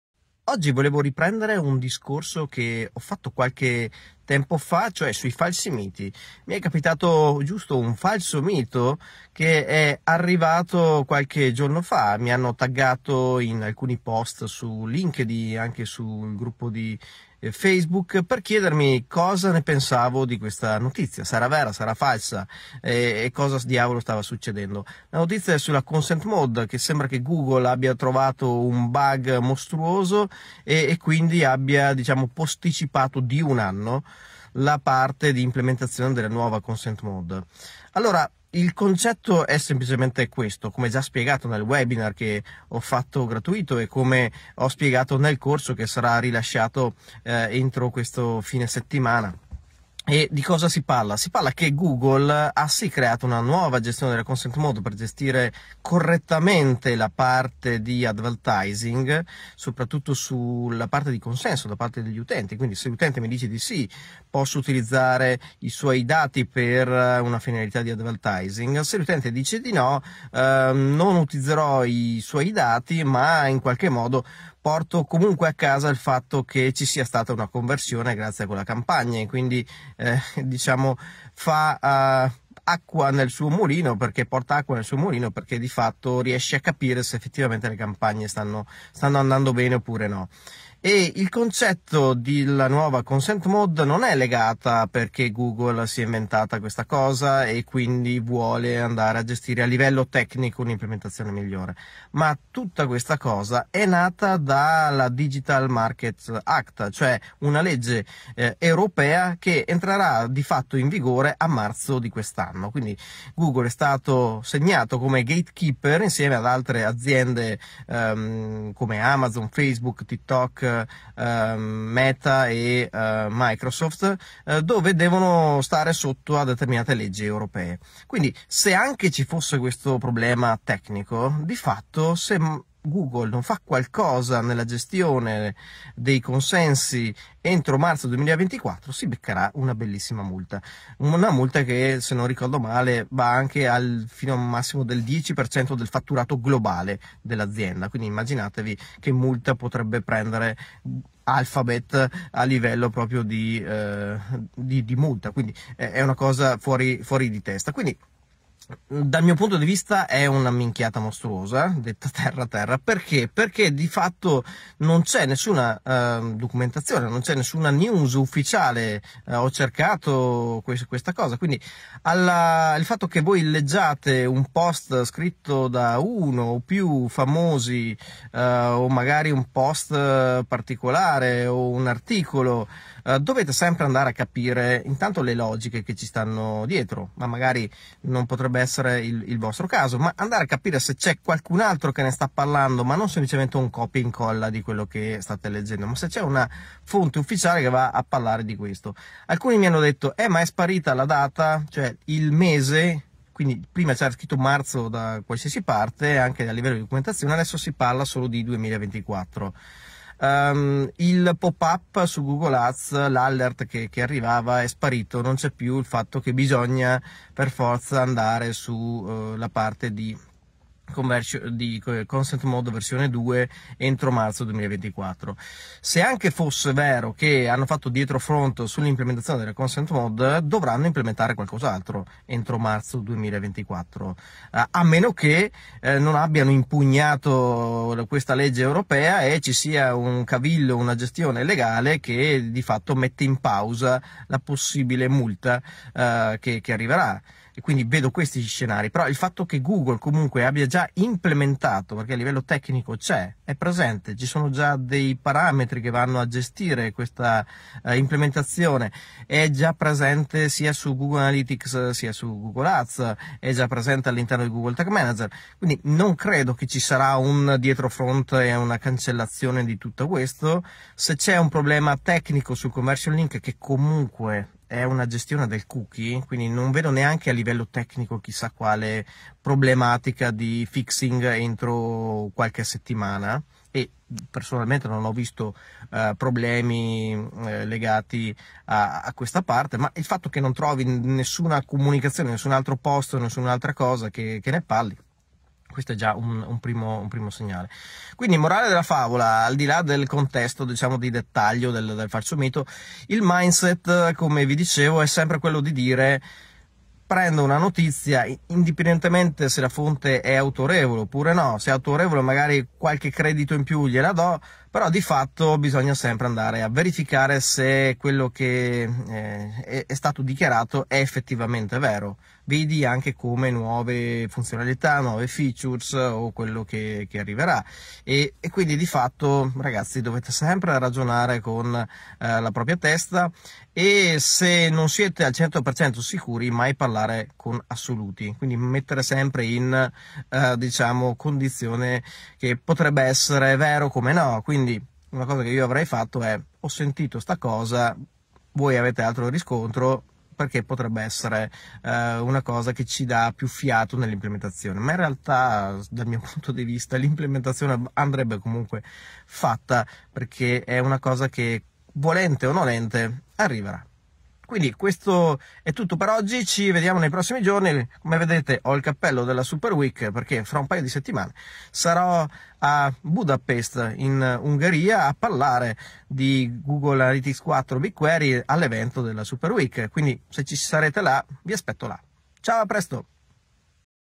Attenzione a questo nuovo “falso mito” sulla Consent Mode v2
Qualche giorno fa il nostro Matteo Zambon (Beta tester e Alpha tester di Google | Co-Fondatore di Tag Manager Italia) si è imbattuto nella presunta notizia - al momento - non sostenuta da fonti ufficiali né da riferimenti o discussioni pubblici…

… riguardo una possibile posticipazione della data di scadenza entro cui è necessario aggiornare alla nuova ConsentModev2 i propri sistemi di tracciamento e gestione privacy.

In questo video fa chiarezza sulla questione.

Come abbiamo spiegato all’interno del webinar gratuito che abbiamo tenuto qualche settimana fa e come mostriamo all'interno del corso dedicato, il concetto è:

→ non aggiornare i propri progetti di digital analytics alla nuova Consent Mode v2 significa azzoppare il ritorno sull'investimento e i risultati delle proprie campagne Google Ads...

... perché non si potranno usare i dati delle conversioni dei propri annunci per creare campagne di retargeting efficaci.

That's it.

Facci sapere nei commenti cosa ne pensi.

Grazie e... buon Tag!

[Link per richiedere l'accesso alla registrazione del webinar gratuito "Rivoluzione Consent Mode v2"]

[Link per sapere di più sul nuovo corso "Clear Consent v2"]